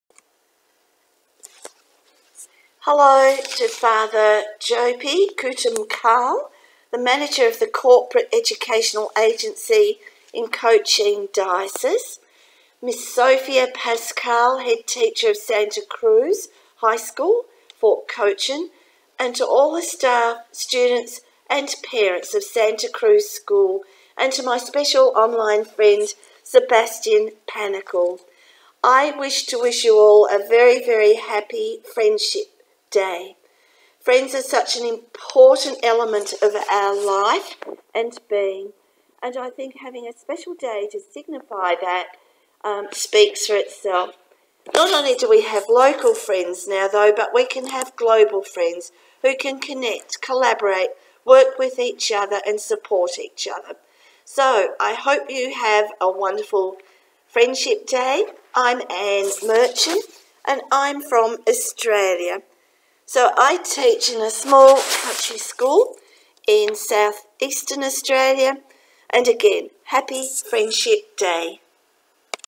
Friendship Day Message from Anne Mirtschin, Australia
Oh what an honor! A Friendship Day message from 'Connector Extraordinaire' Anne Mirtschin, winner of this years Making IT Happen Award!